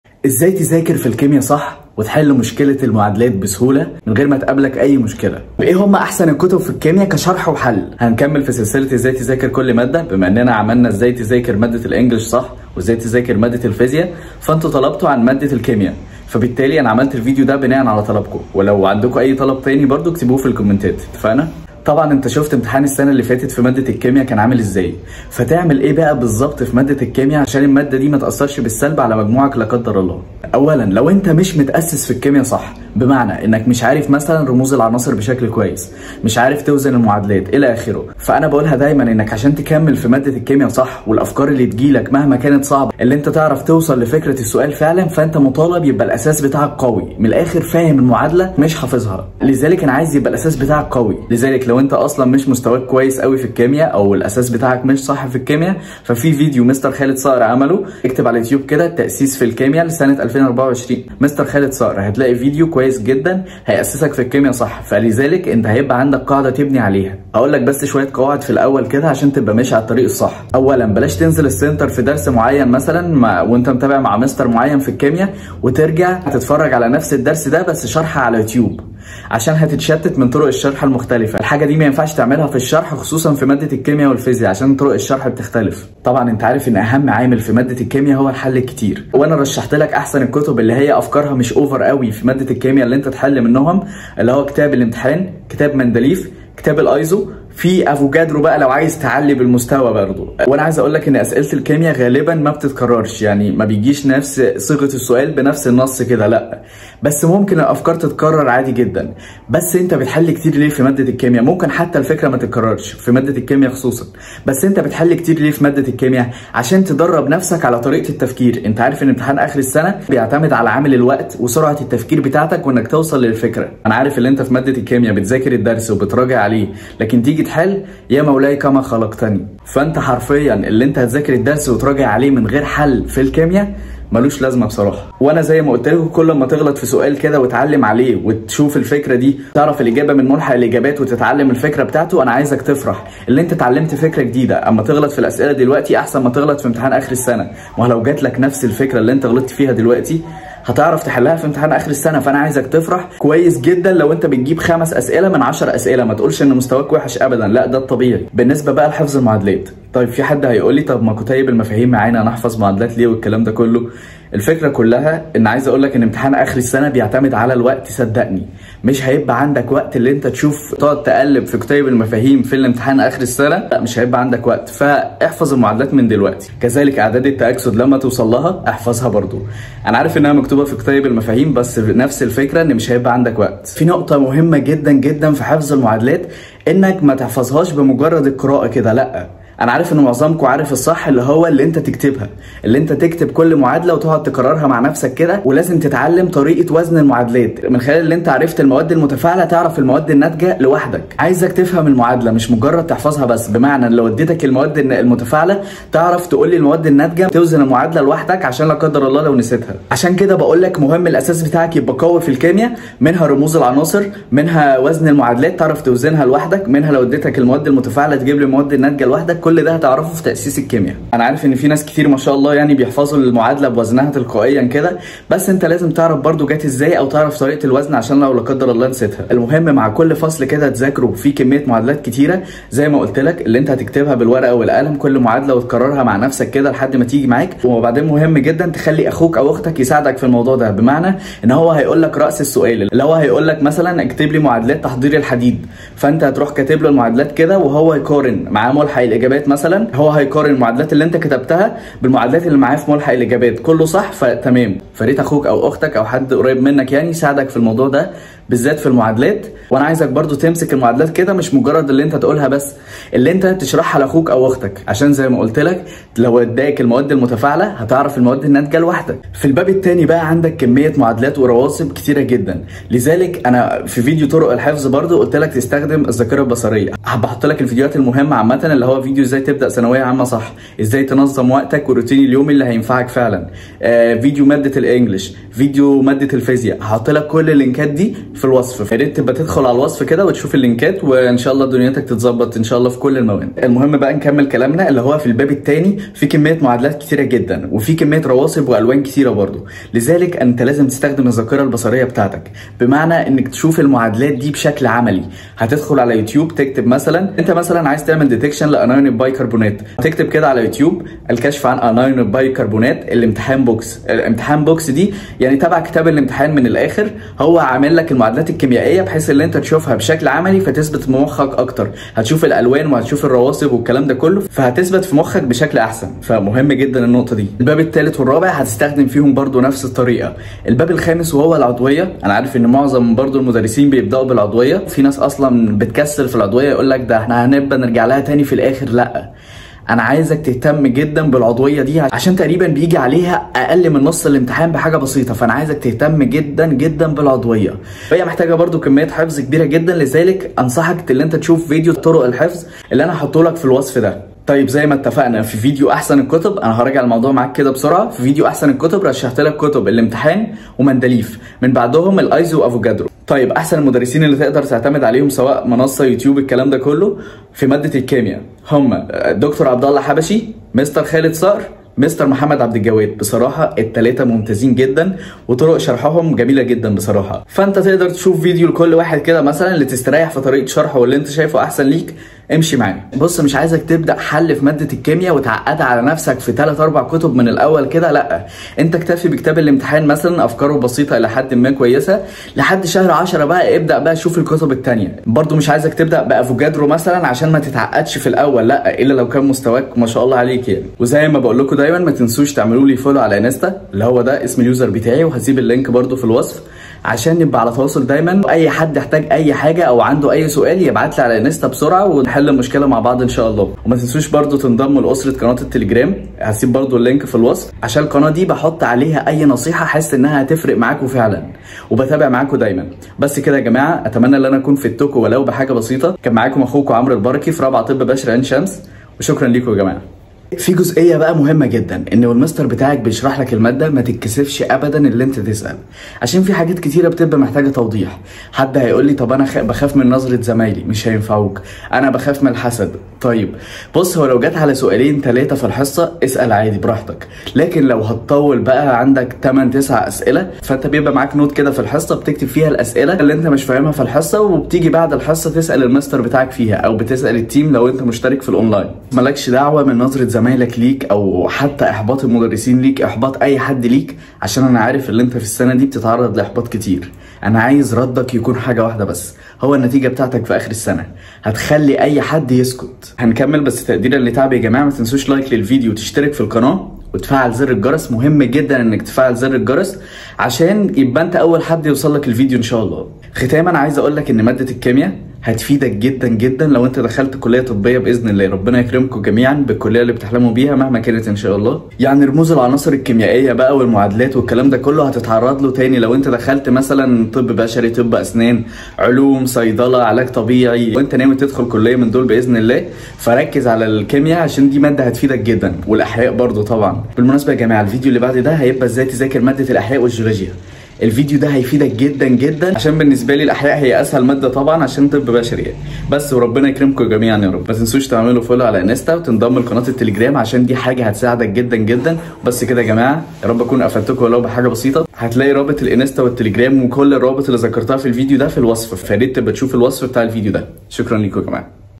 ازاي تذاكر الكيمياء صح بالتفصيل!!طريقة حفظ المعادلات بكل سهولة!افضل الكتب والمدرسين في الكيمياء!
ازاى تذاكر الكيمياء صح بالتفصيل وطريقة حفظ المعادلات الكيميائية بسهولة
افضل الكتب والمدرسين في الكيمياء للثانوي
متنسوش تعملولي فولو على انستا عشان لو احتاجت حاجة تكلمني : amr_elbarakyy